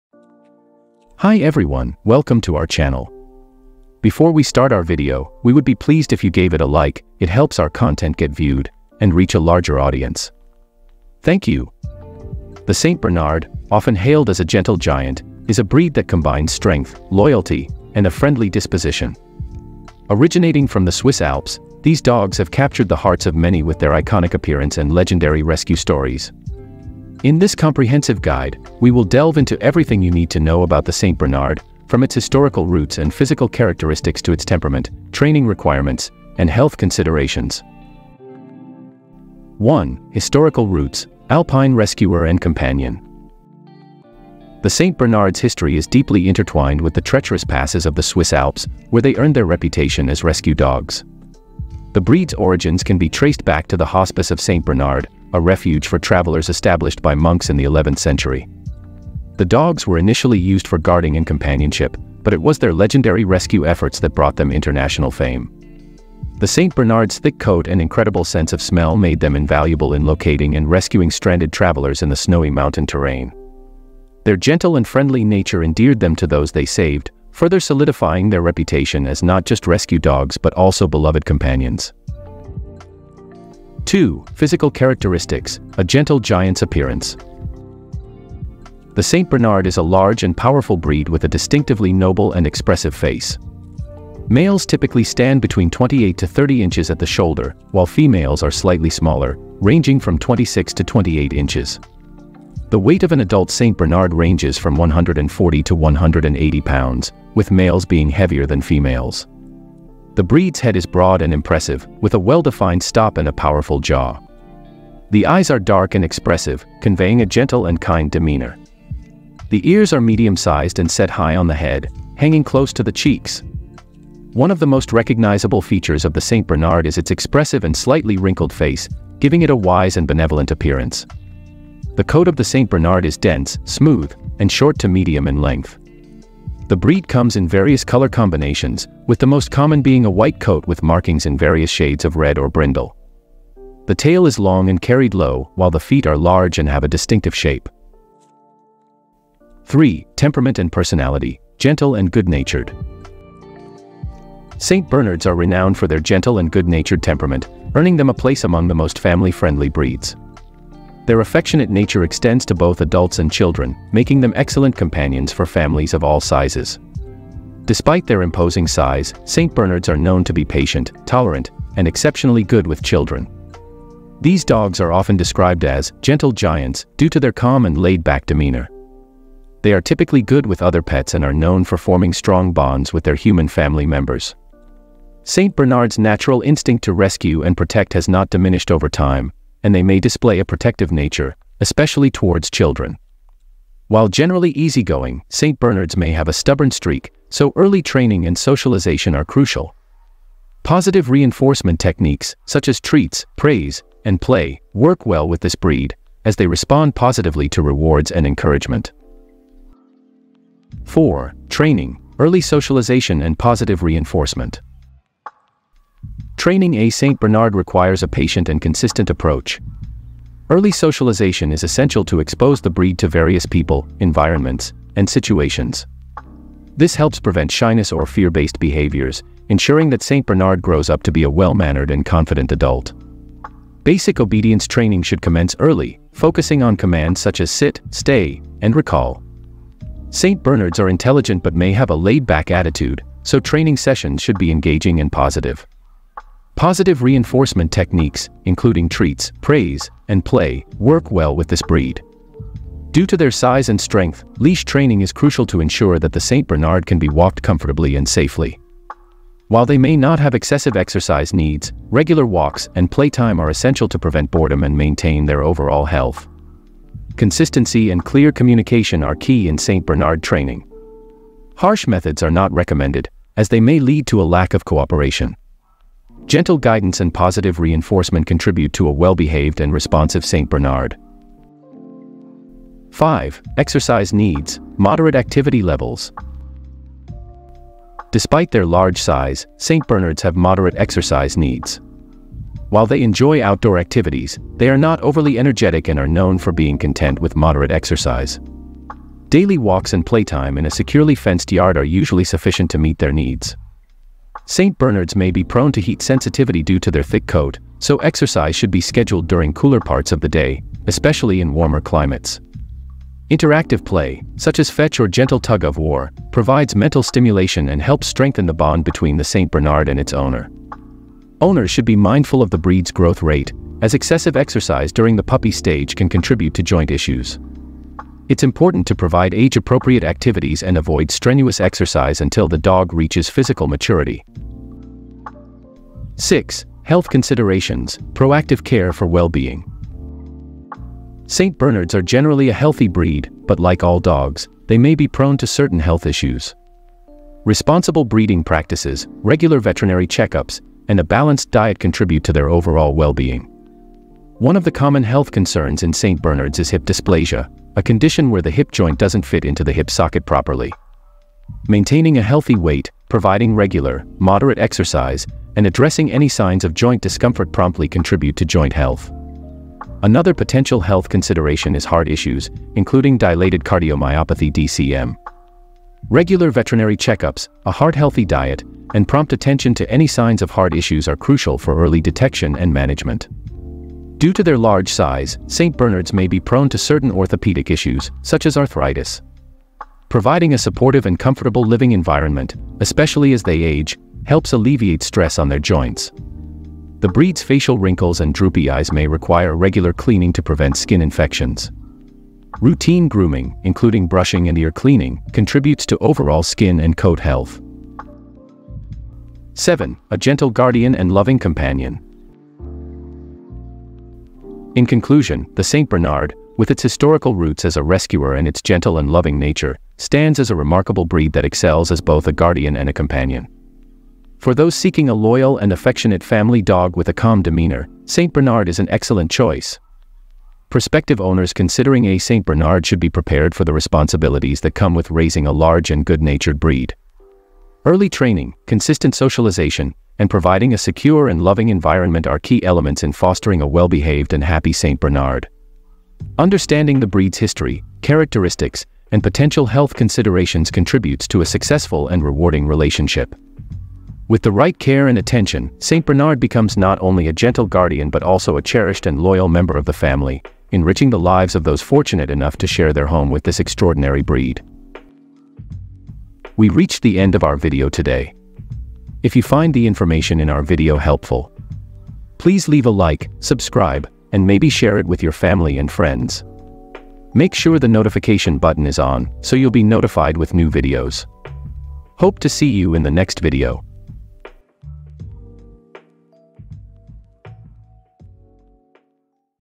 Saint Bernard 101 | Everything You Need To Know About This Gentle Giant.
🏔️ Join us on a journey into the world of the lovable and gentle giant, the Saint Bernard, in our latest video, "Saint Bernard 101." If you're enchanted by massive yet warm-hearted dogs, you're in for an insightful adventure! Discover everything you need to know about the Saint Bernard breed, celebrated for its size, friendliness, and heroic history.

In this comprehensive guide, we'll cover:

Saint Bernard Origin and Heritage
Distinctive Physical Characteristics
Temperament and Family-Friendly Traits
Training Tips for Saint Bernard Puppies and Adults
Grooming and Care Essentials
Health Considerations for Saint Bernard

Whether you're already a Saint Bernard enthusiast or considering this breed for its companionship and gentle nature, this video is filled with valuable insights to help you understand and appreciate the exceptional qualities of this remarkable giant.

Here's to the warmth and charm of the Saint Bernard! 🐶❤️🏡